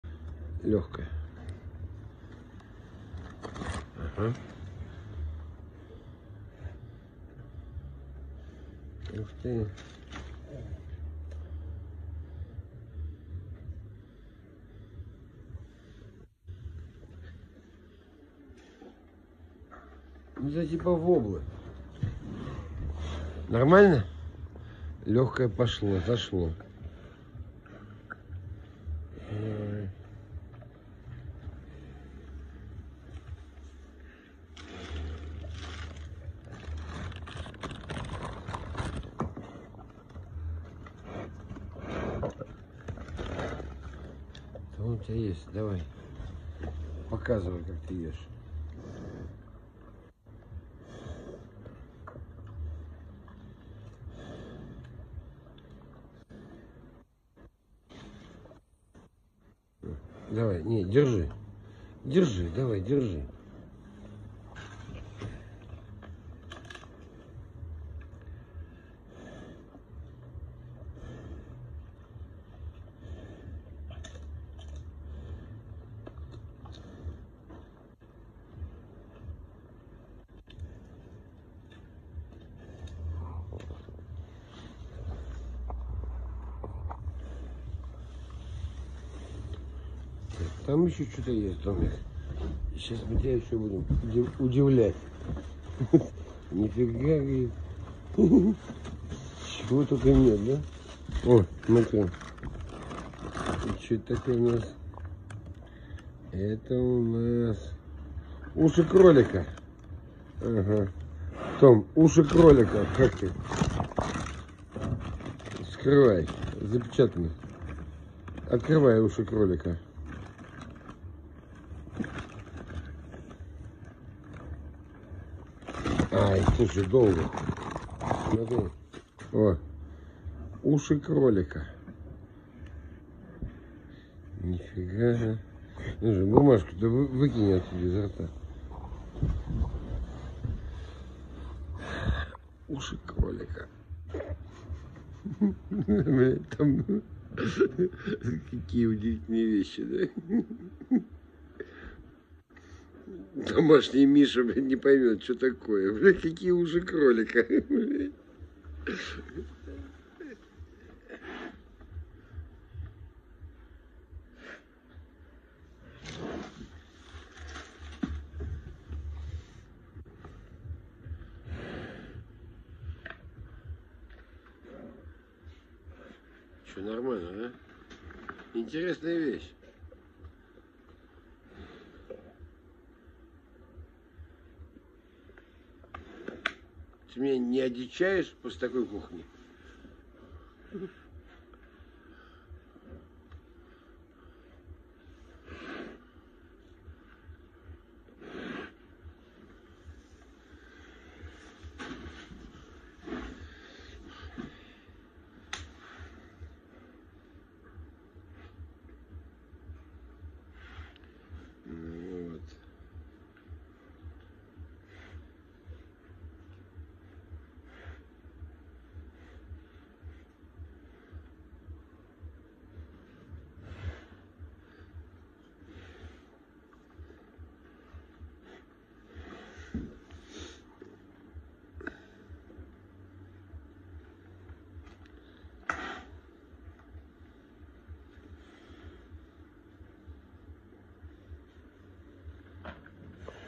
Про домашнего медведя. Pavel Vyakin
★Мир гармонии и искусства с ручным, бурым медведем!
★Путешествие по процессу дрессировки и заботы о животных.
★Видео о том, как строится связь  между человеком и бурым медведем.
★Интересные факты о характере и поведении этих величественных созданий.

Воплощаю любые идеи съёмок.